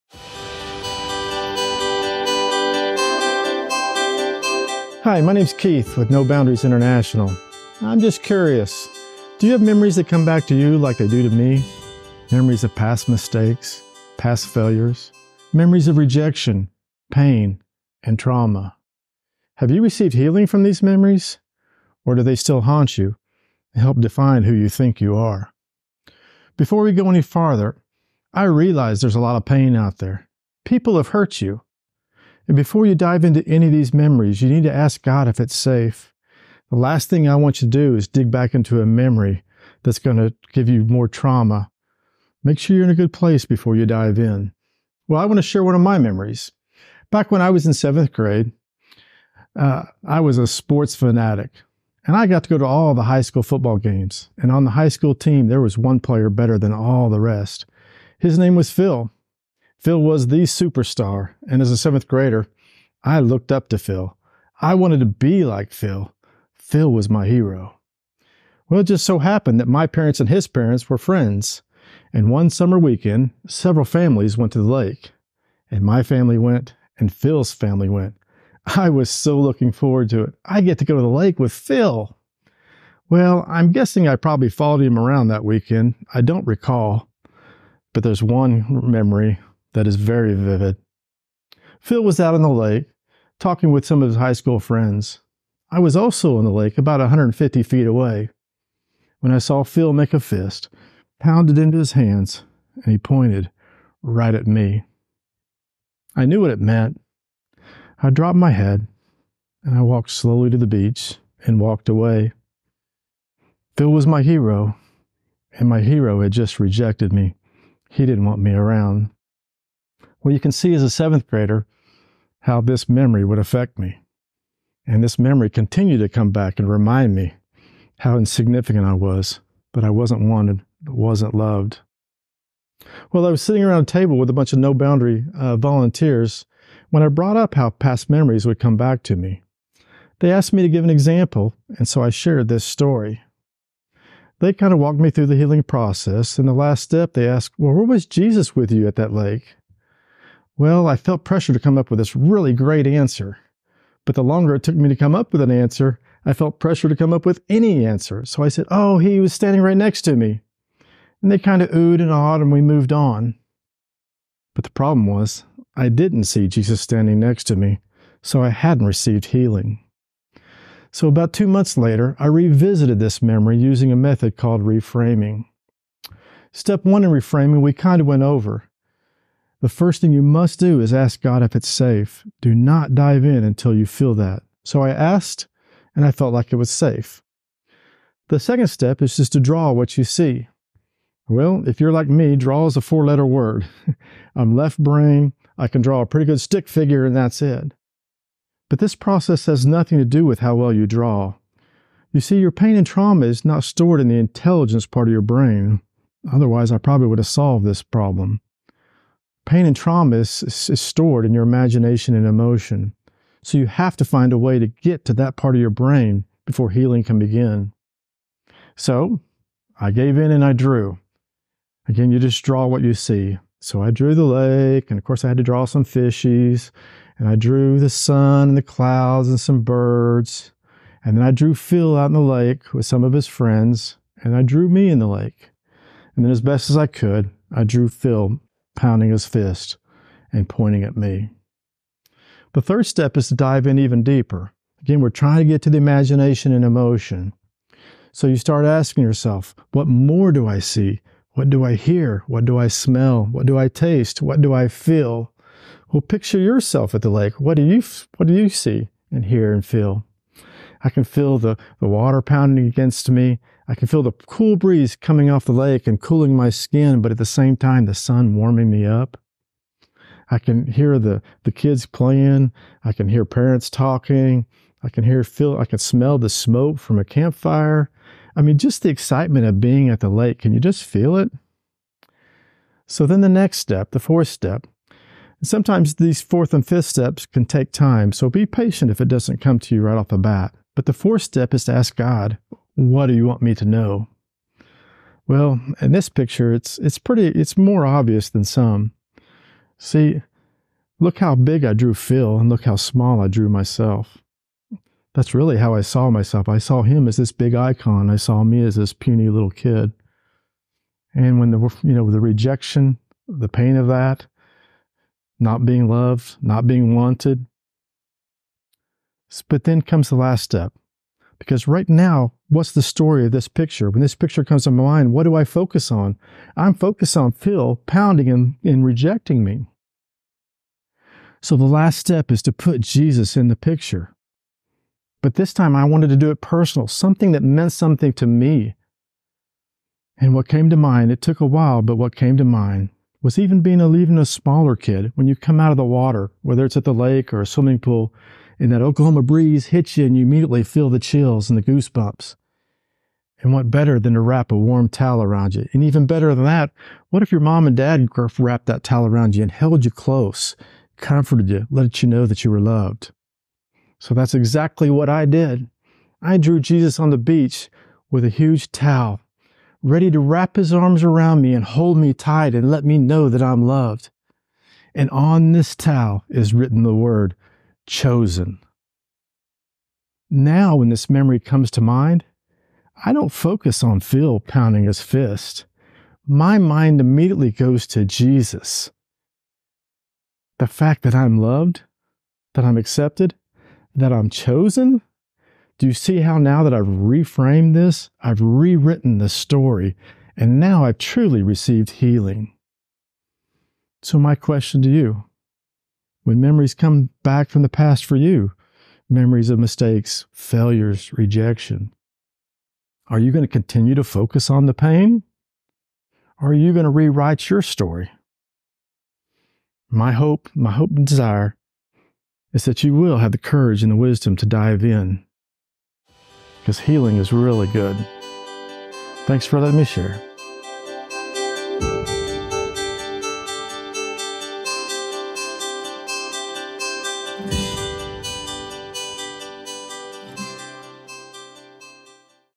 Restoration Moment: "Reframing Rejection" with Keith McGinty
Do you think that only super-difficult, big traumatic events are the only things that can affect your life and your thoughts? Watch Keith as he tells his story of a simple childhood vacation encounter that plagued him for years and how he found healing. To learn how you can change your thoughts and your brain, watch our Journey of Restoration: CPR Style video e-course--found right here on our YouTube channel!